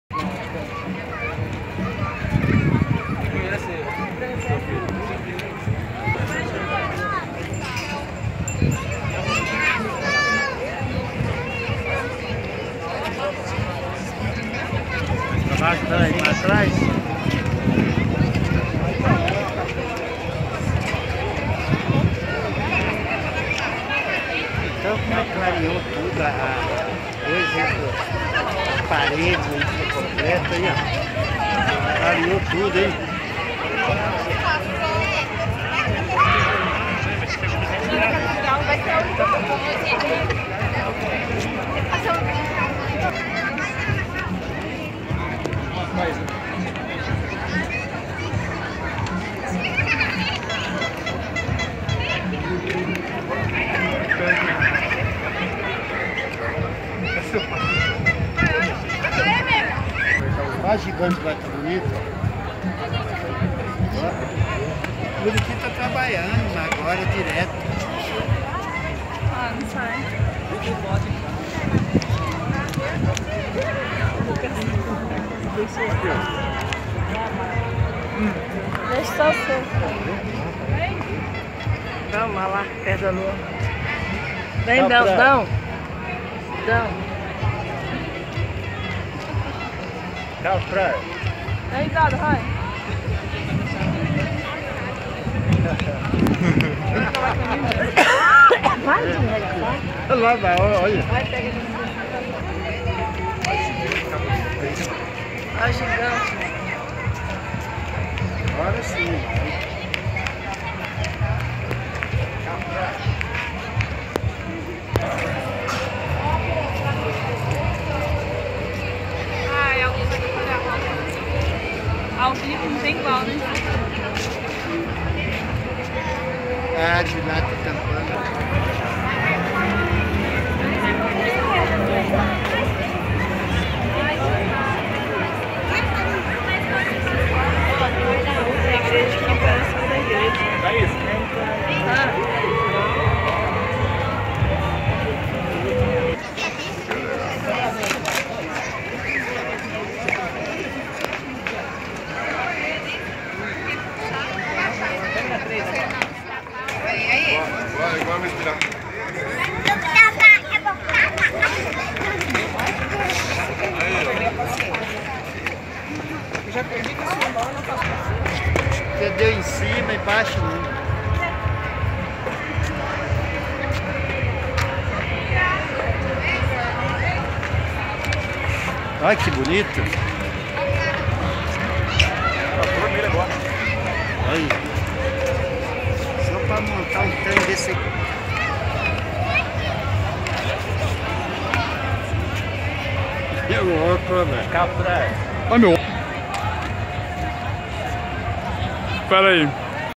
Maringá Encantada - Natal 2018
Um rápido giro pelas decorações de Maringá, Catedral Metropolitana e ao seu redor.